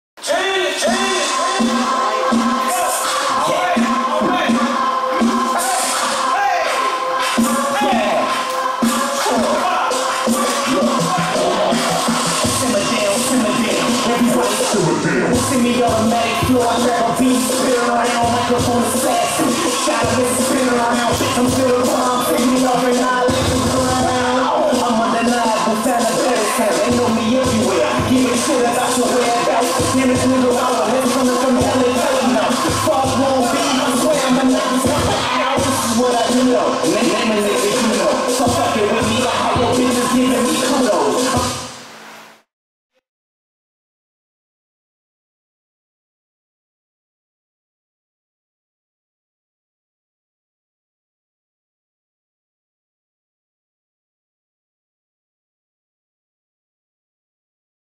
Sparkin' it Out | Sierra Neudeck | Choreography -- Shane Sparks
Sierra Neudeck

Taught at Millennium Dance Complex in San Fernando Valley, CA

Dancers - Jordyn Jones, Sierra Neudeck

Next Generation Dancers

DanceOn Network